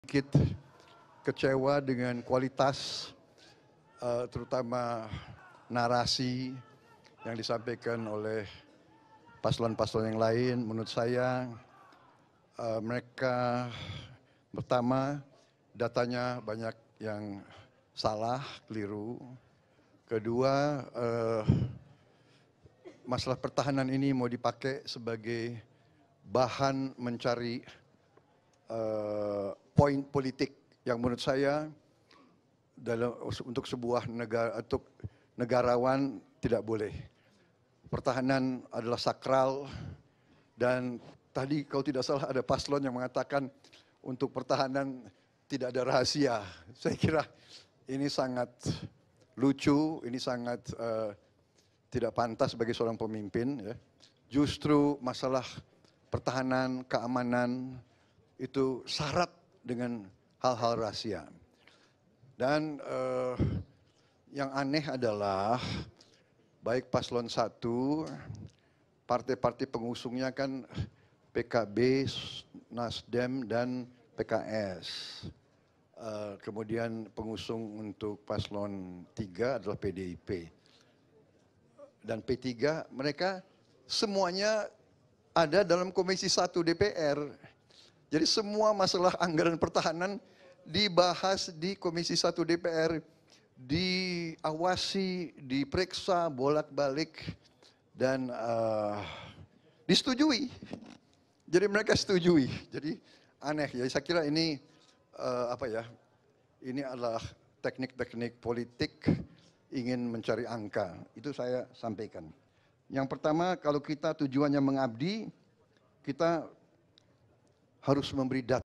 PRABOWO TERANG-TERANGAN MENGAKU KECEWA DENGAN DEBAT KETIGA
Bagaimana menurut BiNetizen?
Yuk komentar di bawah!!!